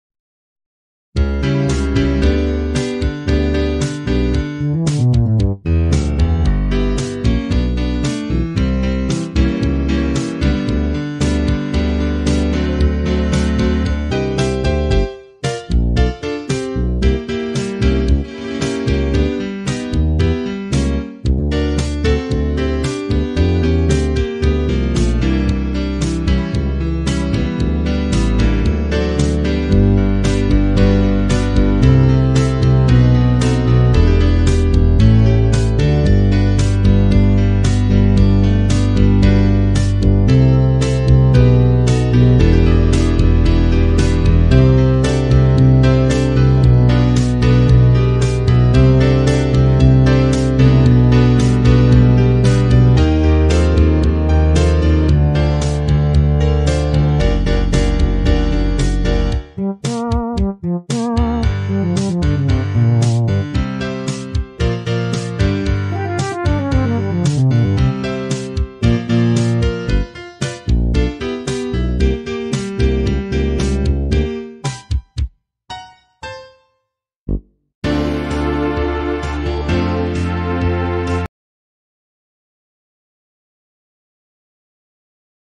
Strange Man with Bi-colored Mustache playing Vulfpeck [Re-recorded with better audio/Transcribed]
Yes, I notated that entire thing by hand because I, myself, am strange. :3
For those who are wondering-- the musician here is actually Charly Garcia.